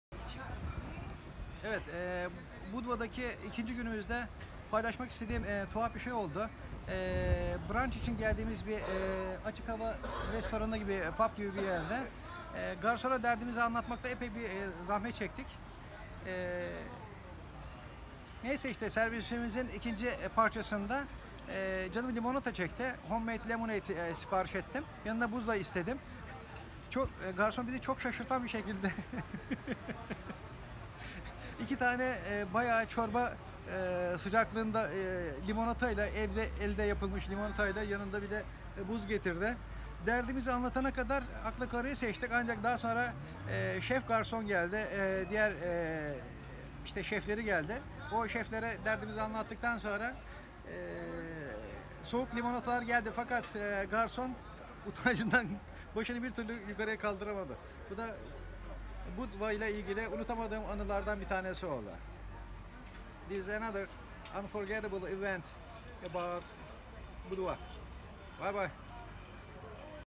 Hot Lemonade!
We ordered cold lemonade and extra ice cubes, the waiter brought me a glass of hot lemonade and a glass of ice cubes! This happened in Budva , Montenegro, in this summer. LOL...